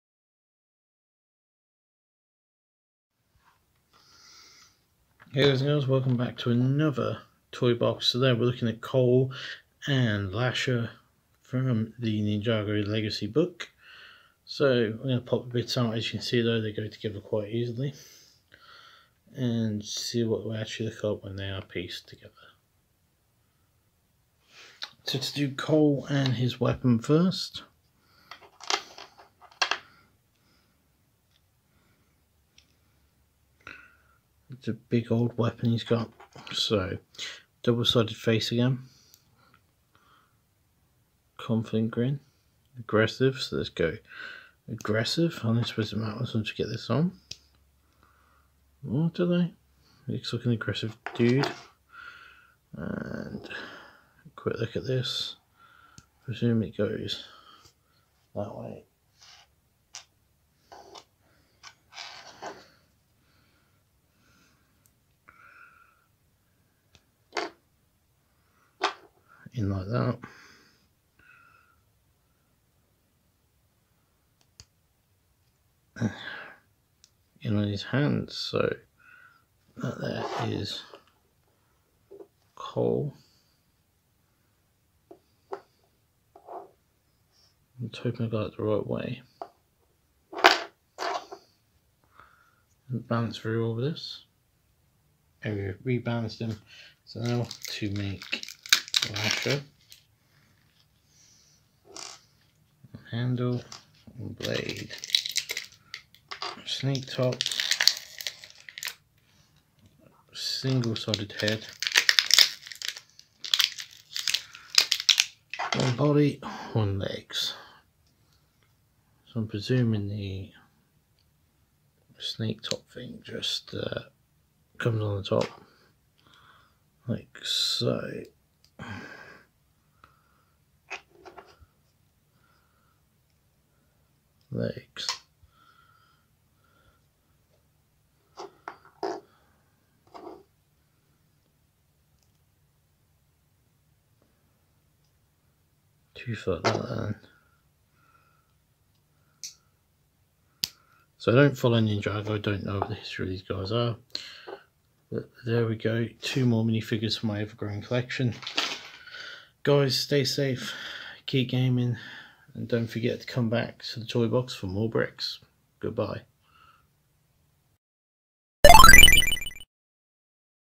Lego Ninjago Cole and Lasha
Here are 2 really cool Ninjago Figures from the Ninjago Legacy magazine.